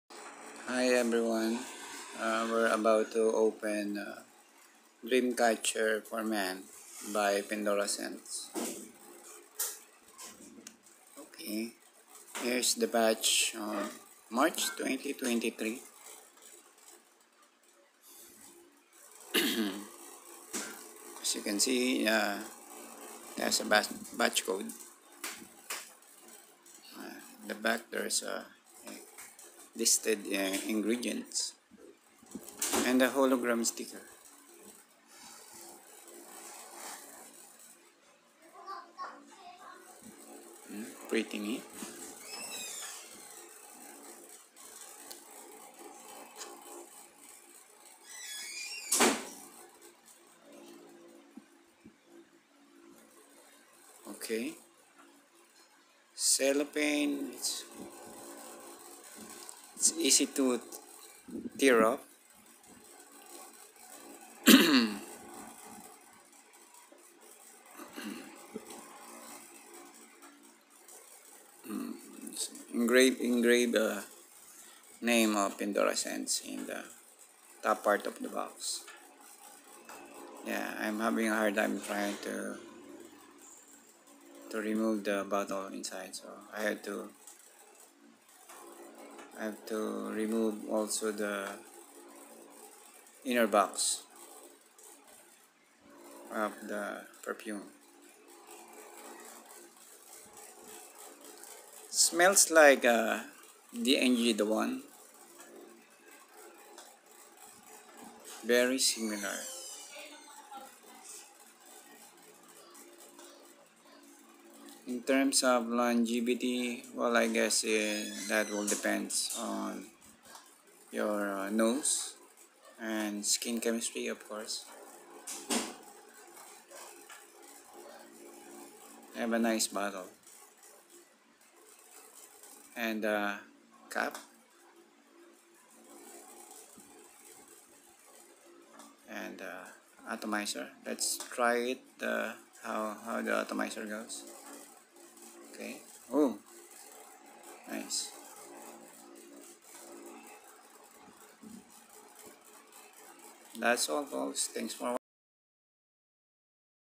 DREAM CATCHER for men by Pendora Scents (Batch 03/2023)
Fragrance Notes:

Top: Grapefruit, Coriander and Basil

Middle: Ginger, Cardamom and Orange Blossom

Base: Amber, Tobacco and Cedar